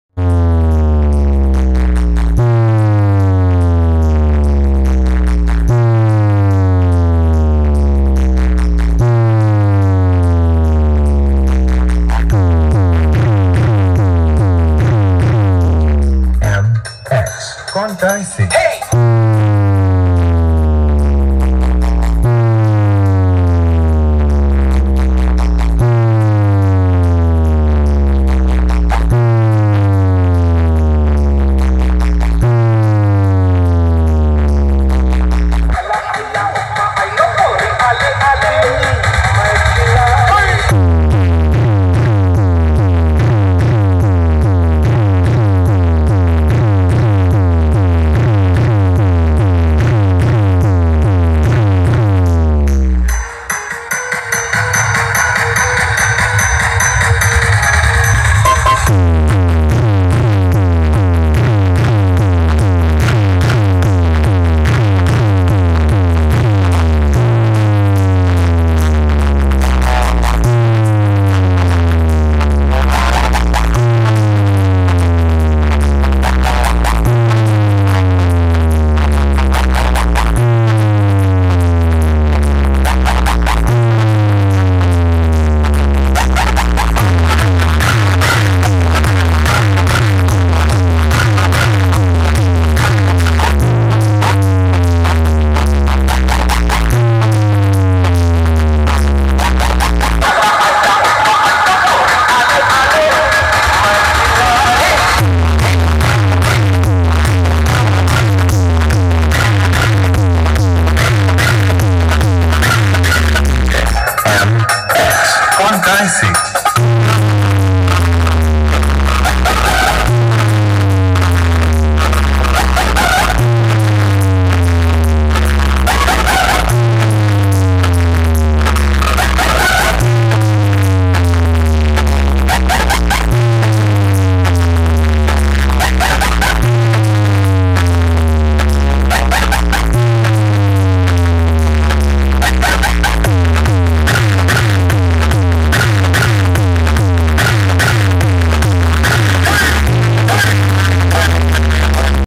এই প্রথমবার 18" পপ ব্যাস দেখলাম পশ্চিম মেদিনীপুর এ রেজাল্ট কেমন (শালবনী কমলা কম্পিটিশন)
এই প্রথমবার 18" পপ ব্যাস দেখলাম পশ্চিম মেদিনীপুর এ (শালবনী কমলা কম্পিটিশন) @boxduniya